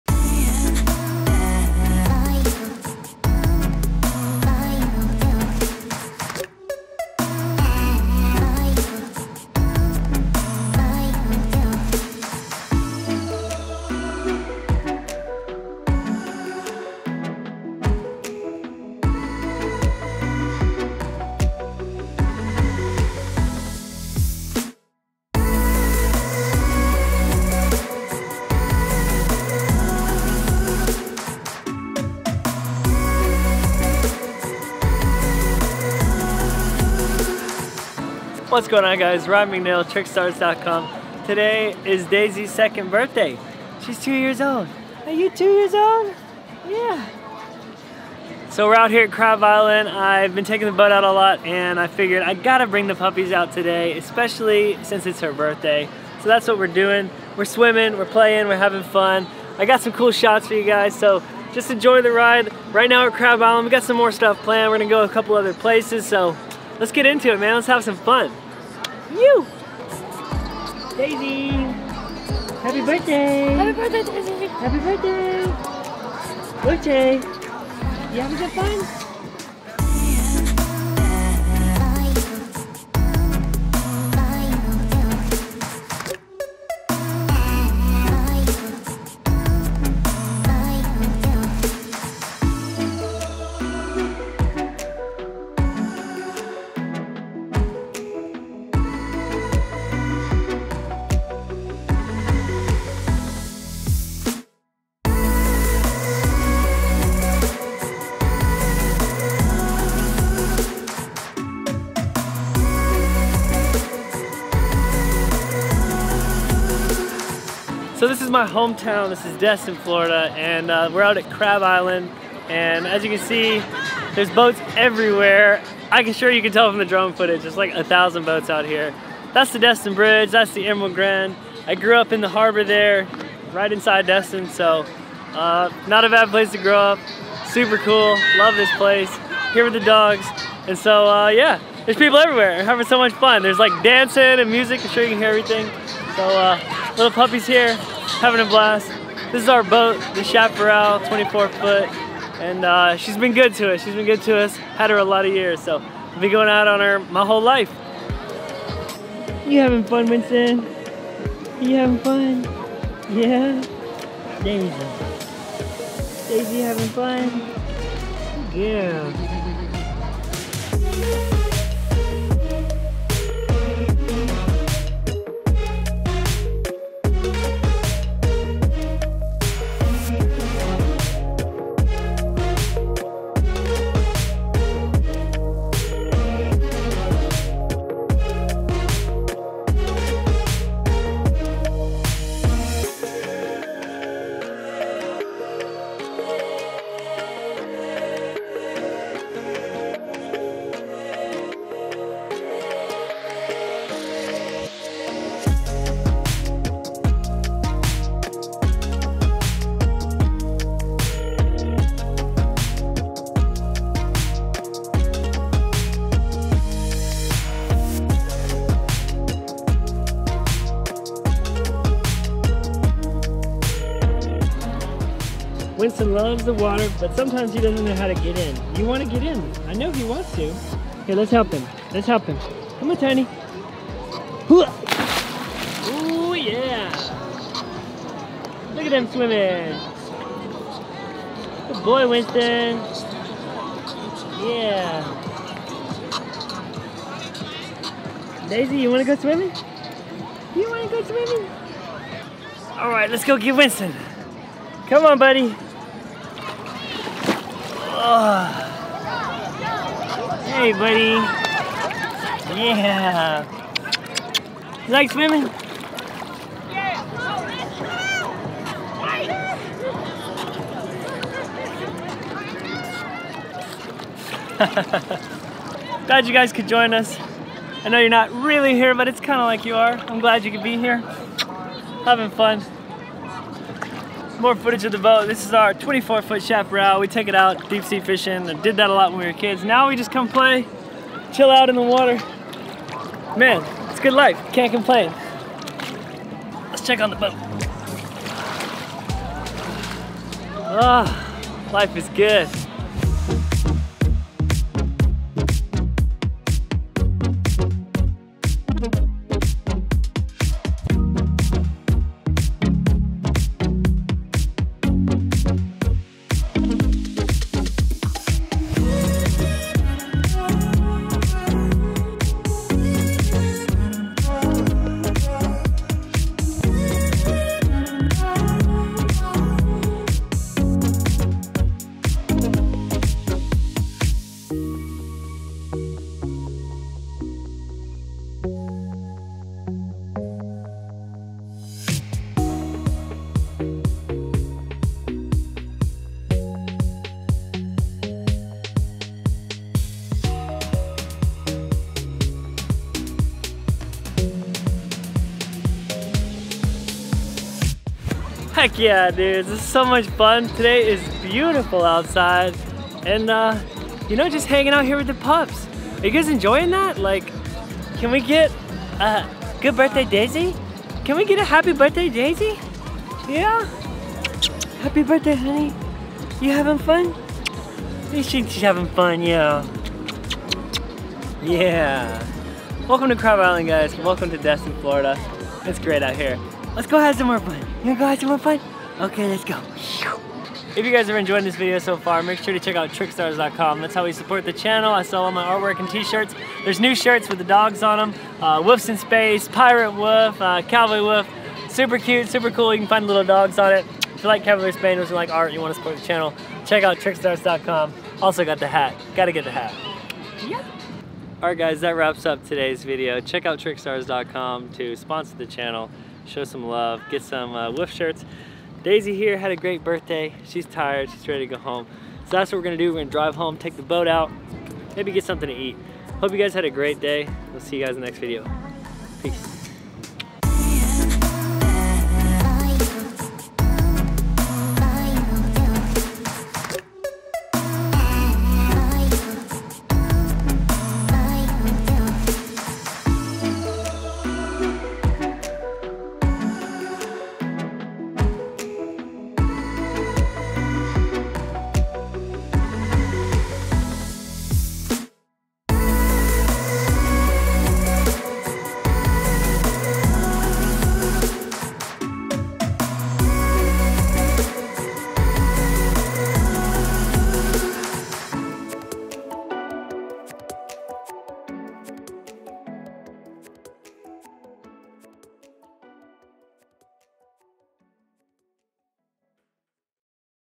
Puppy Island - Cavalier Spaniels in Destin Florida
Cavalier King Charles Spaniels swimming at Crab Island in Destin Florida! They had so much fun celebrating Daisy's 2nd Birthday! This is my favorite spot in Destin Florida, Crab Island!